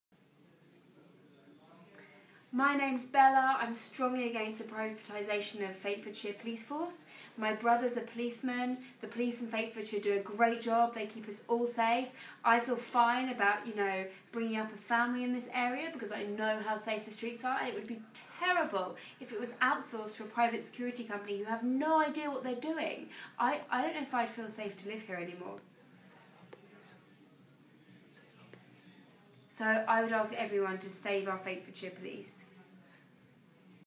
SaveOurBobbies
Local residents speak out against the privatization of the police force.